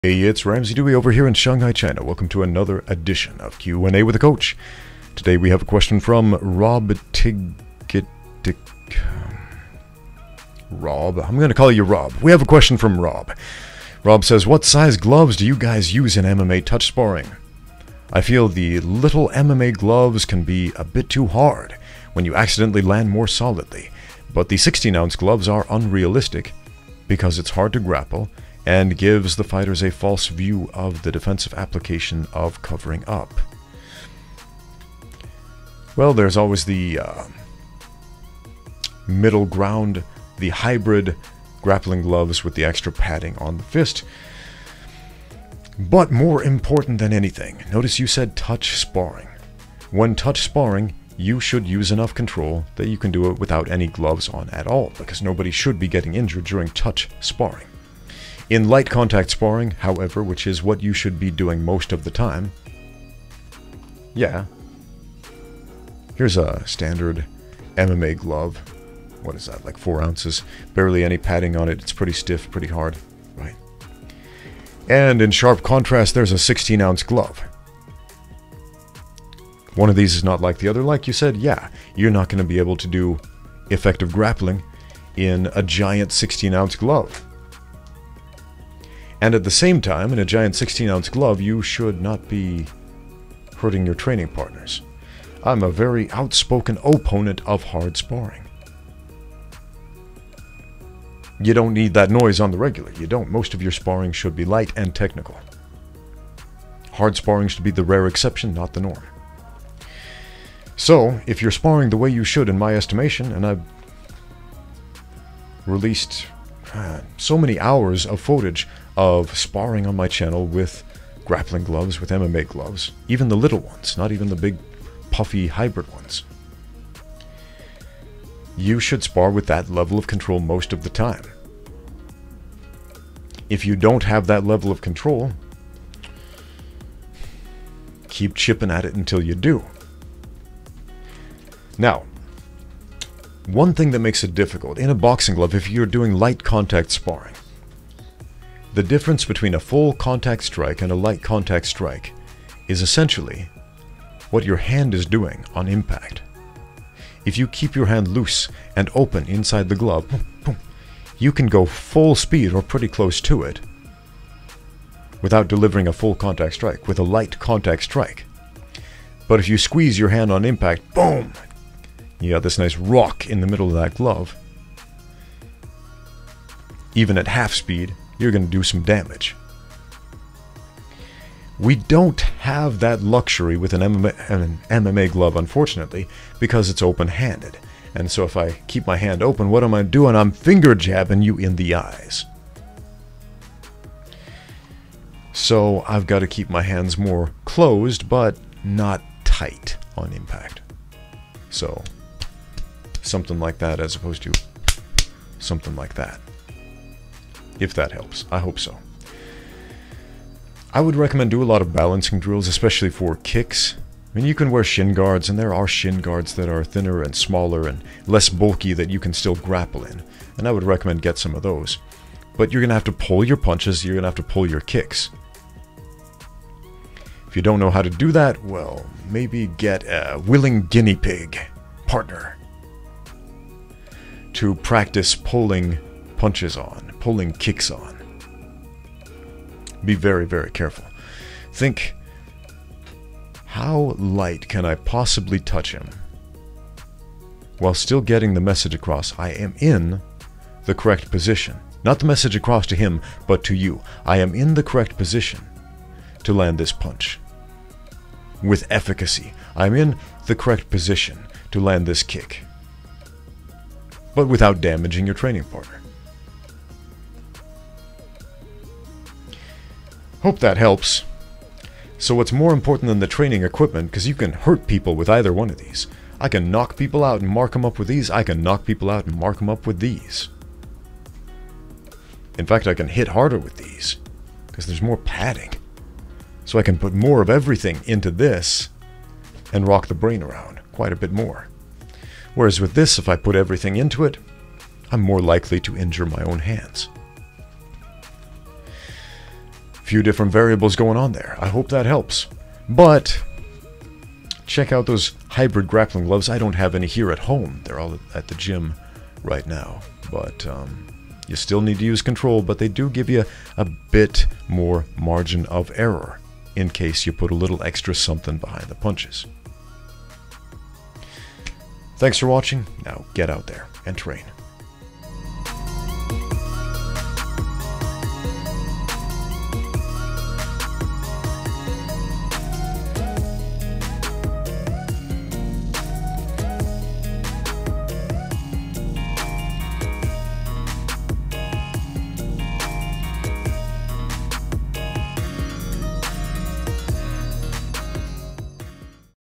You don’t need giant gloves for sparring, you need CONTROL
Q&A with the coach. Shanghai based MMA coach and Kunlun Fight Combat League ringside commentator Ramsey Dewey answers questions from the viewers.

How can you do MMA sparring safely with 4 ounce grappling gloves? Should you use hybrid MMA gloves instead? Should you do MMA sparring n in boxing gloves? Could it be possible that perhaps, just maybe, you should learn to use a higher level of control so you don’t injure your training partners?